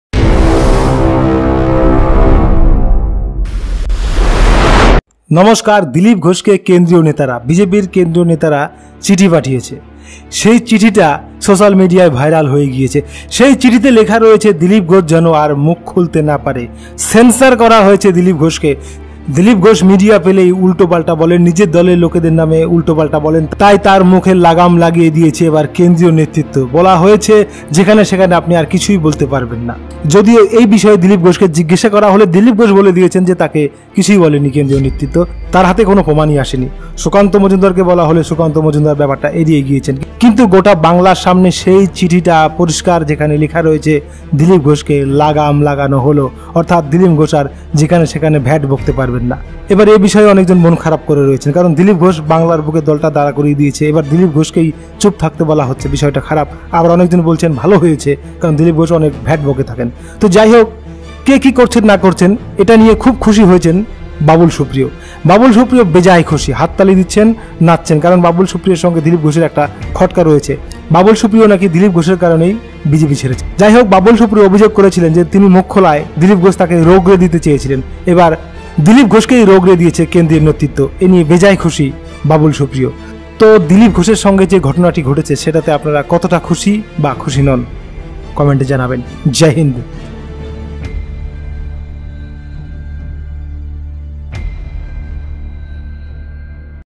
dilip ghosh/দিলীপকে চাপে রাখায় বেজায় খুশি বাবুল
দিলীপ ঘোষকে কেন্দ্রীয় নেতৃত্ব চিঠি. দেওয়ার খবর প্রকাশ্যে আসার পরে পরেই আক্রমণ করেছিলেন বাবুল সুপ্রিয়। মঙ্গলবারই দিলীপকে

.খোঁচা দিয়ে টুইট করেন বাবুল। .সেই আক্রমণের জবাব দিতে চান না .জানাতে গিয়েও বাবুলকে আক্রমণ করে বসলেন দিলীপ ঘোষ। .

বললেন, ‘‘বাবুল রিজেক্টেড মাল। ওঁর কথার আবার কী জবাব দেব।’’ এর সঙ্গে দিলীপের আরও সংযোজন, ‘‘জাতীয় লিগের খেলা ছেড়ে উনি এখন স্থানীয় লিগে খেলতে এসেছেন।’’.